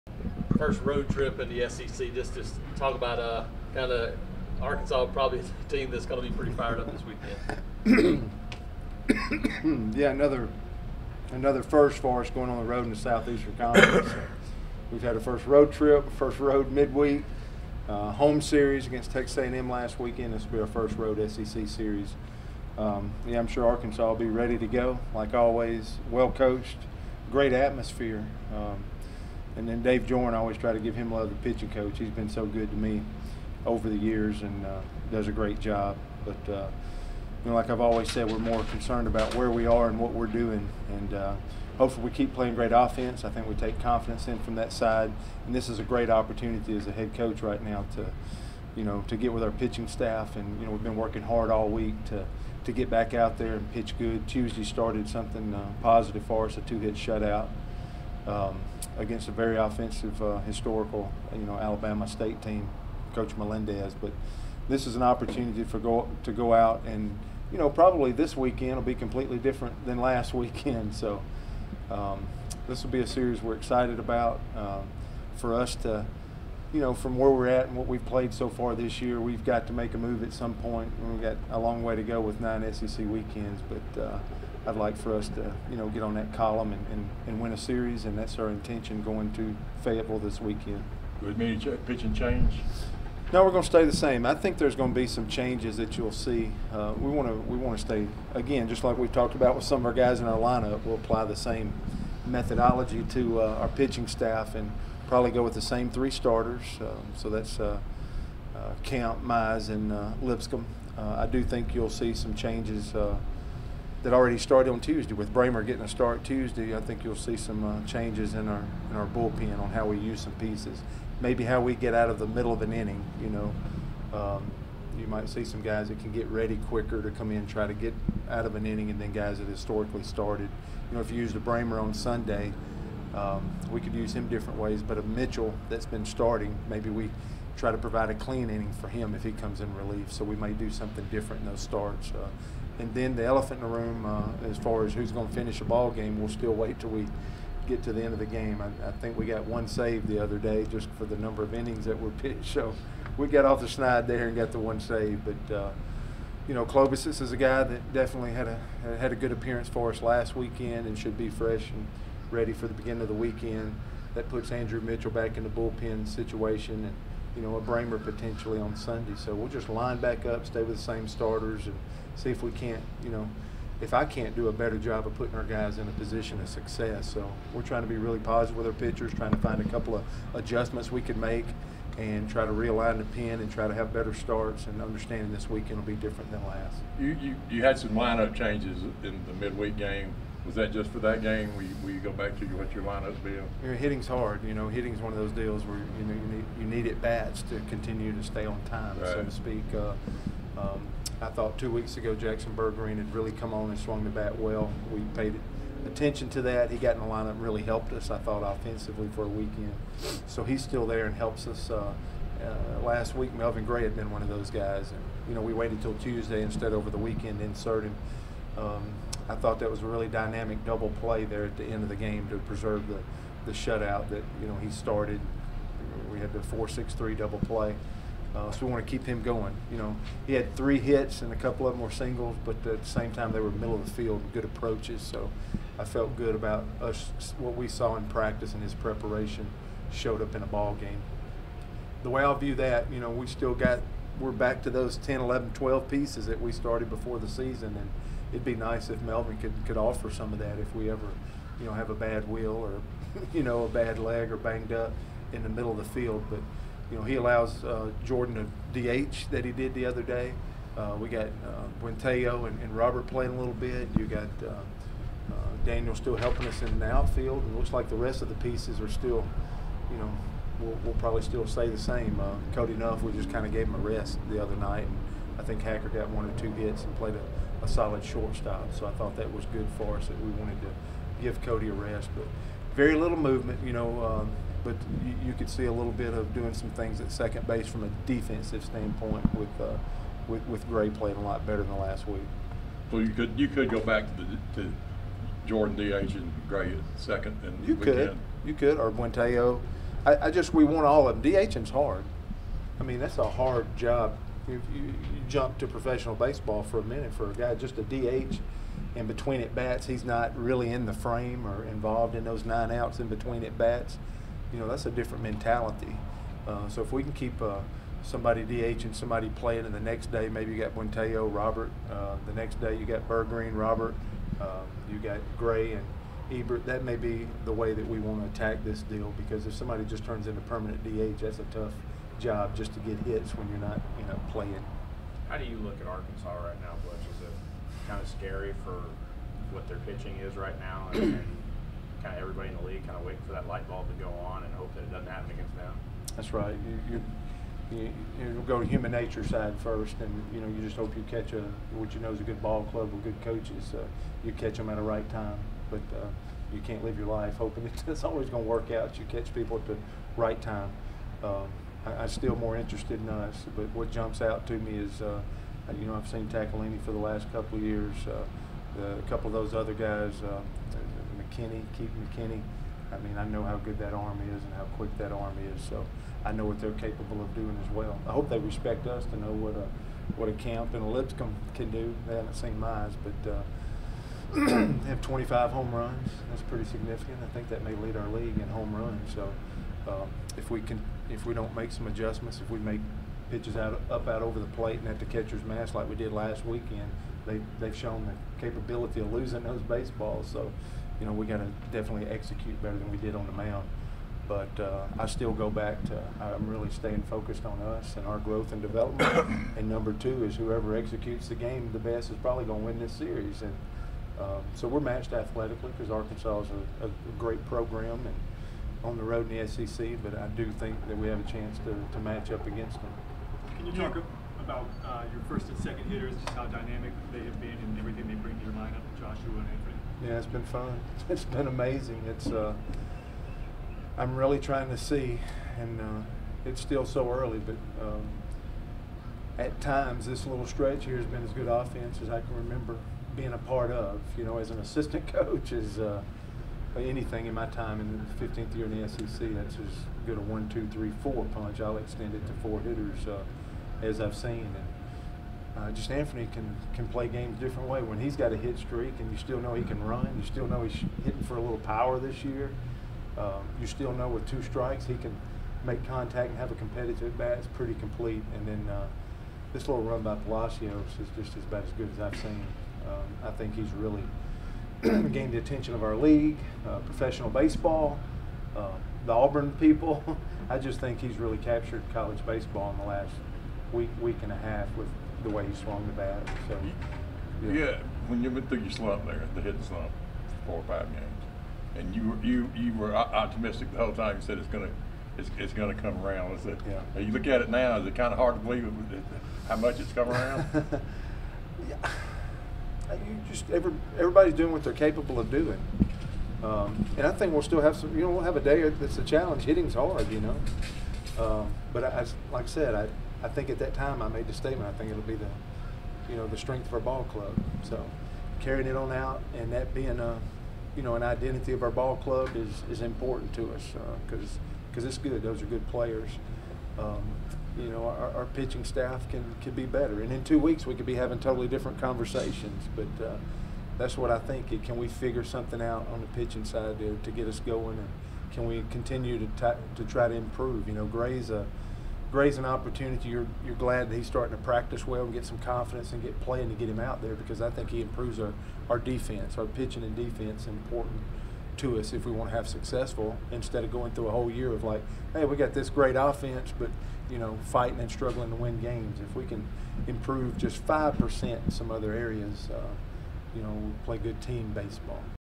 Butch Thompson previews Arkansas series
Auburn baseball coach Butch Thompson previews the Tigers&apos; first SEC road trip of the season: a three-game series with the Arkansas Razorbacks. (Courtesy Auburn Athletics)